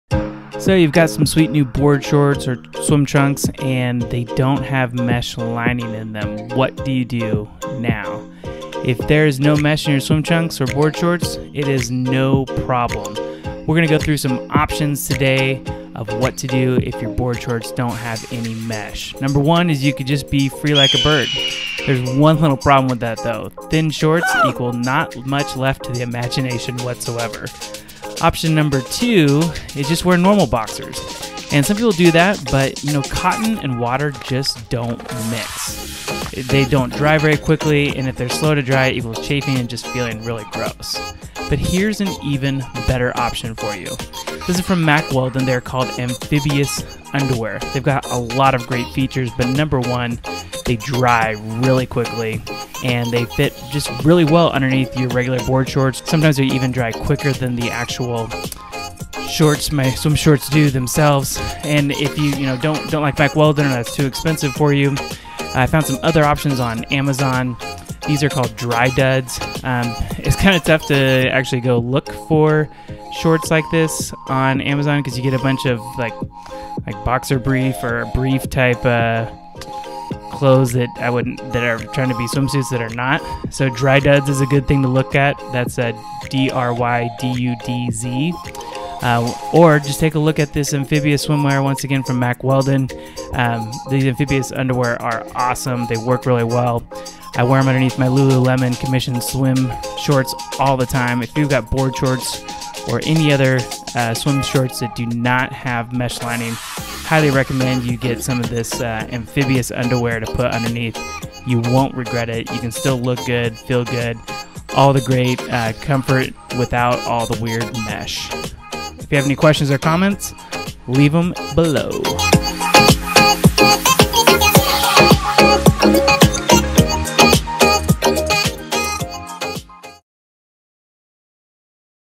What do you wear under board shorts? The age old mystery solved.
You've got some sweet board shorts and noticed, "These don't have any mesh lining!!!" What do you wear under board shorts without mesh lining? What about: what do you wear under swim trunks that don't have any mesh lining?

We answer that age-old question with several different options. Watch our video about what to wear under board shorts to find out what you've been dying to know, but are afraid to ask: what do people really wear under board shorts?

Many popular swim trunks (like some of the new Lululemon Swim Trunks, our review don't have built-in underwear or mesh lining. So what do you do?